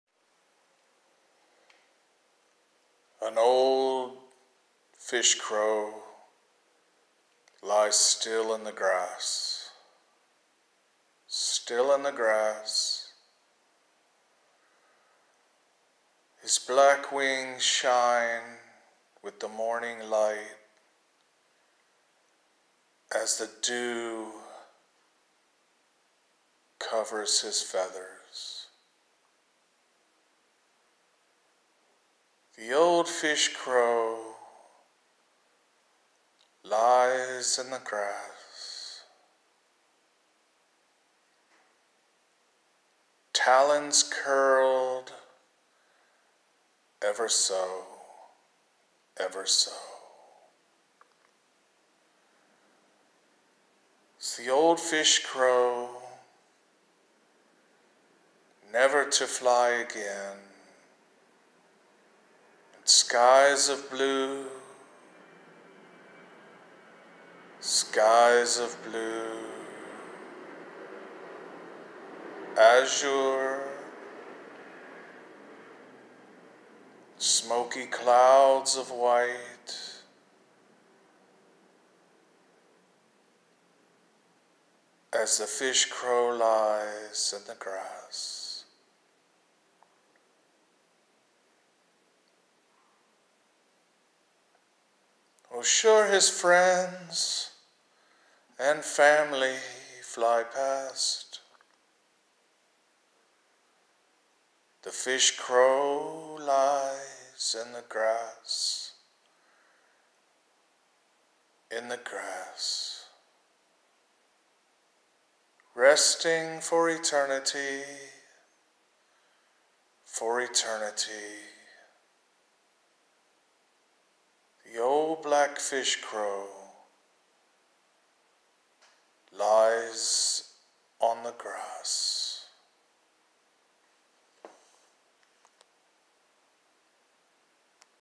ode to a fish crow
poem written by joe swamp dawg henry, spoken word by joe swamp dawg henry as well.